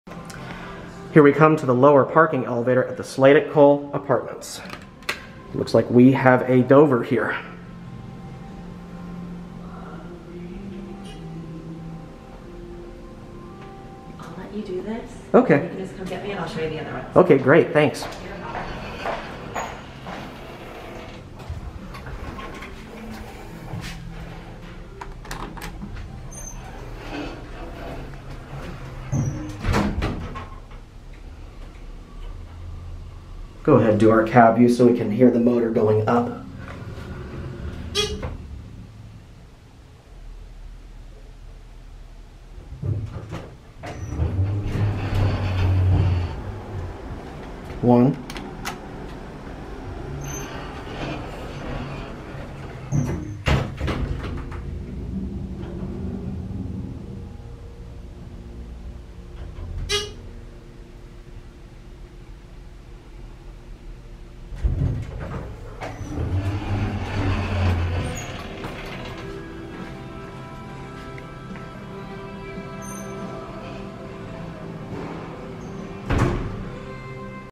Dover Hydraulic Lower Parking Elevator At Slate At Cole Apartments
Riding the lower parking garage elevator at Slate At Cole Apartments in Dallas TX